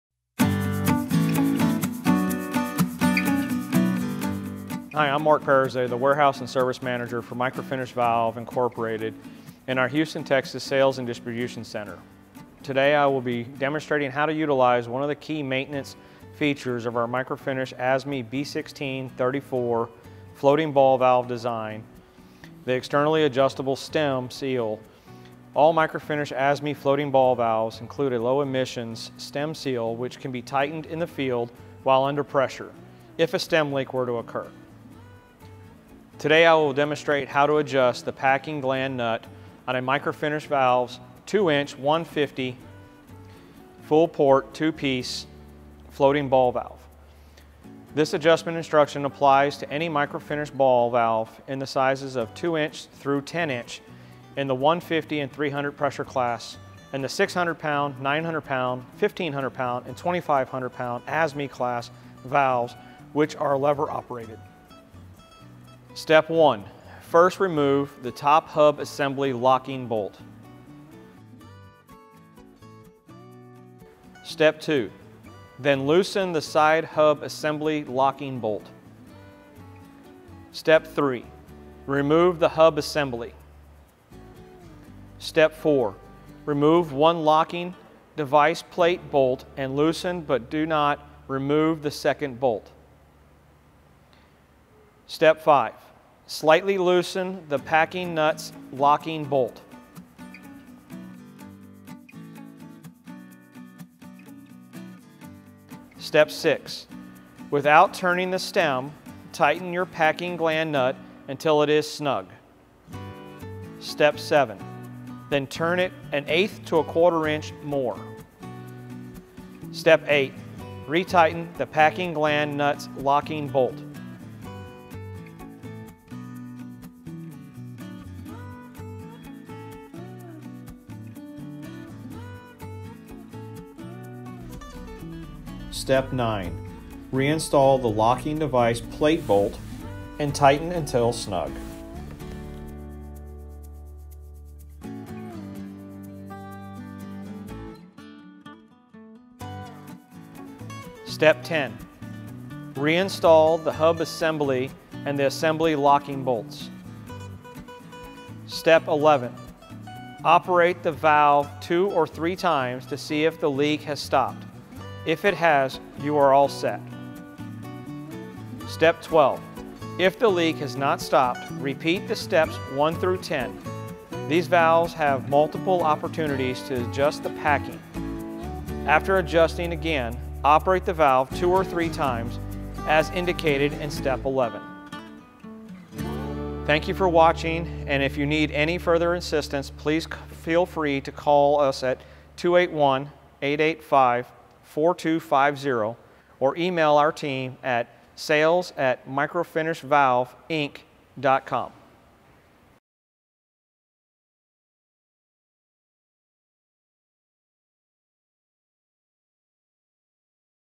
2” 150 Full Port Ball Valve Packing Gland Nut Adjustment Demo
Hi I’m Marc Pariza, the Warehouse and Service Manager for Microfinish Valves, Inc. in our Houston, Texas Sales and Distribution center. Today I will be demonstrating how to utilize one of the key maintenance features of our Microfinish ASME B16.34 floating ball valve design, the externally adjustable stem seal. All Microfinish ASME floating ball valves include a low emission stem steal which can be tightened in the field, while under pressure, if a stem leak were to occur. Today I will demonstrate how to adjust the packing gland nut on a Microfinish 2” 150# Two Piece full port floating ball valve. This adjustment instruction applies to any Microfinish Floating Ball Valve in sizes 2” thru 10” in the 150# and 300# pressure classes and all 600#, 900#, 1500# and 2500# ASME class valves which are lever operated.

1. First remove the top hub assembly locking bolt.

2. Then loosen the side hub assembly locking bolt

3. Remove the hub assembly

4. Remove one locking devise plate bolt and loosen but do not remove the 2nd bolt.

5. Slightly loosen the packing gland nut’s locking bolt

6. Without turning the stem, tighten your packing gland nut until it is snug

7. Then turn it 1/8” to ¼” more.

8. Re-tighten the packing gland nut’s locking bolt.

9. Re-install the locking devise plate bolt and tighten until snug.

10. Re-install the hub assembly and the assembly locking bolts.

11. Operate the valve 2 or 3 times to see if the leak has stop. If it has, you are all set.

12. If the leak has not stopped, repeat steps 1 thru 10. These valves have multiple opportunities to adjust the packing. After adjusting again, operate the valve 2 or 3 times as indicated in step 11.